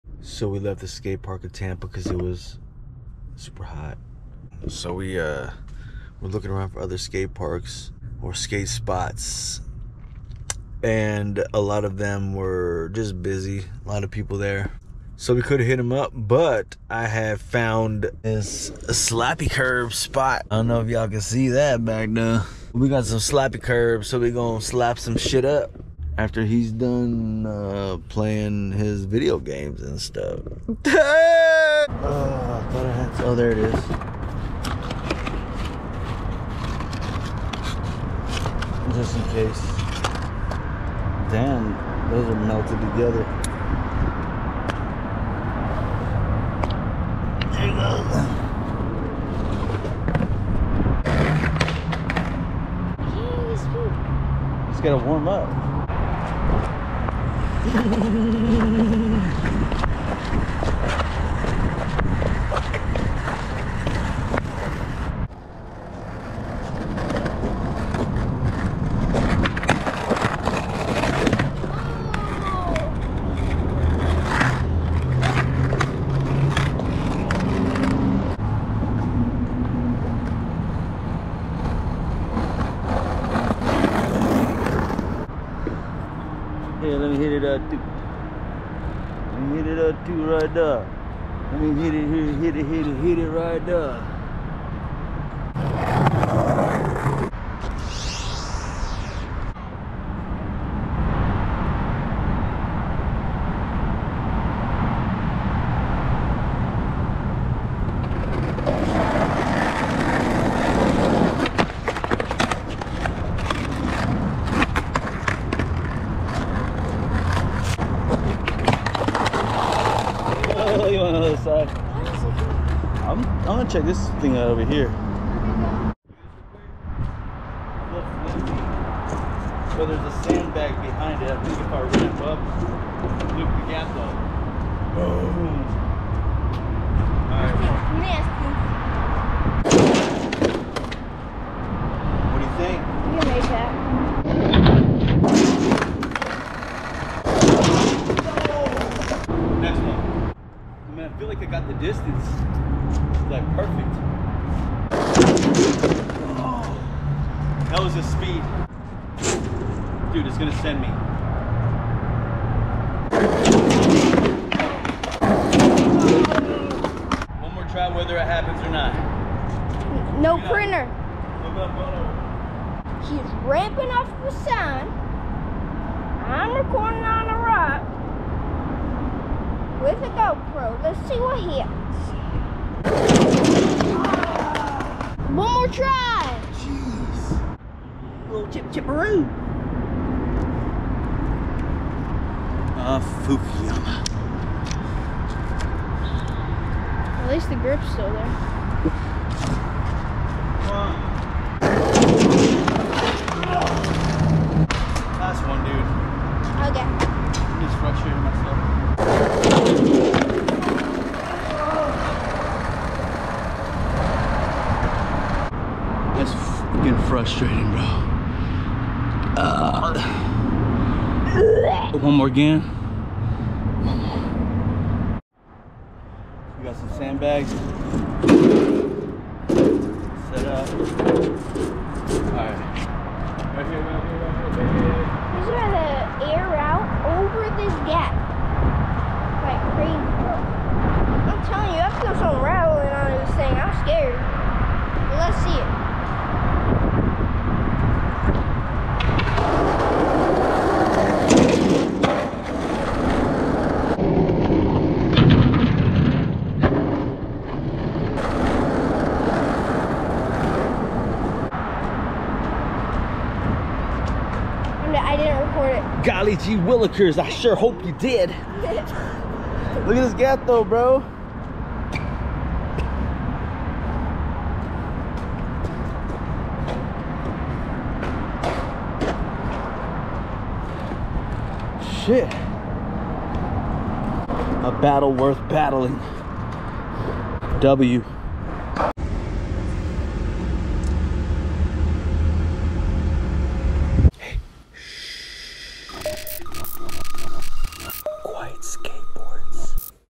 A Battle Worth Battling │ Skate Spot in Tampa │ Father Son Skateboarding │ Quiet Skateboards
So.. was a bit too hot skating indoors at the skatepark of Tampa. Found this dope slappy curbspot not too far around the corner. Curbs here are super nice. Zen caught a couple nice clips on em'.

While filming Zen, I seen this gnarly gap with sign already propped up. So, it was a must to at least try the MF. Felt a nice kick off of it, so it had to be done. LET THE BATTLE BEGIN! lol.

Rough roll up. Sign kicker, super sketchy. Roll away narrow. WAS PERFECT! You can't see it on my face, but I loved every minute of that battle. Frustrating yeah.. but that's what it is. Worth it. I want to kick that biz now!

If you have any clips at this spot, hit us up on Instagram, tag us. We want to see that for sure! Maad respects! On to the next one...

SALUTE!

Looking forward to seeing where the journey takes us next and the people we get to meet and give light to along the way! For the Love of Skateboarding!